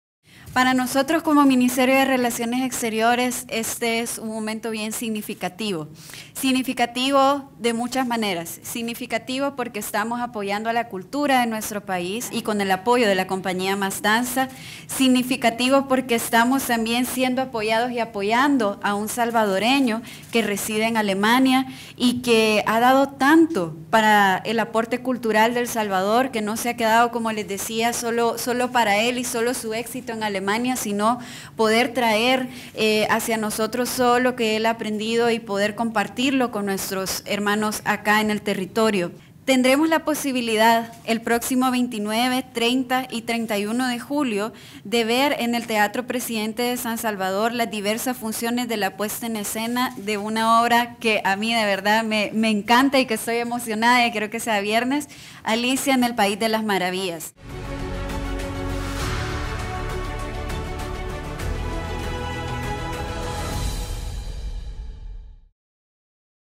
Ministerio de Cultura informa sobre la puesta en escena de Alicia en el País de las Maravillas
AliciaEnElPaísDeLasMaravillas | “Estamos siendo apoyados y apoyando a un salvadoreño que reside en Alemania y que ha dado tanto para el aporte cultural de El Salvador”, expresó la viceministra de Relaciones Exteriores, Adriana Mira.